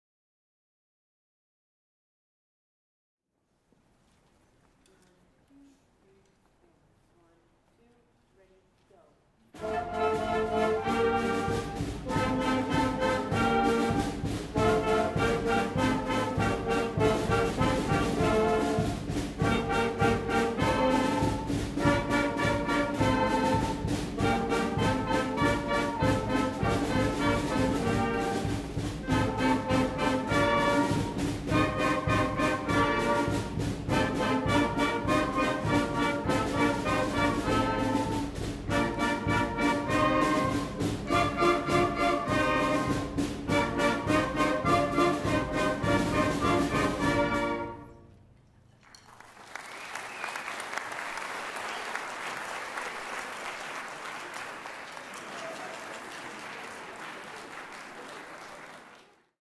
Mahoney Winter Concert - 8th Grade Band warm up
8th Grade warm up performed by the Mahoney Middle School 8th Grade Band at the Winter Concert in South Portland, ME on December 14, 2017